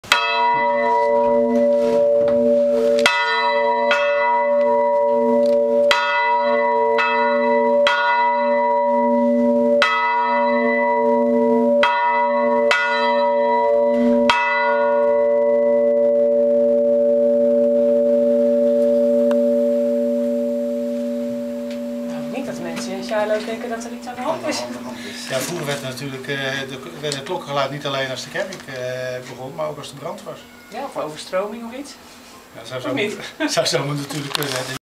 Charloisse klok Oude Kerk
Rotterdam 3-4-2012 Vandaag op pad met Joop van der Hor (deelgemeente Charlois) en Chris Vemer van Radio Rijnmond op zoek naar de mooiste plekjes van Charlois en naklimpartij aanbeland in de kerktoren bij de kerkklokken van Charlois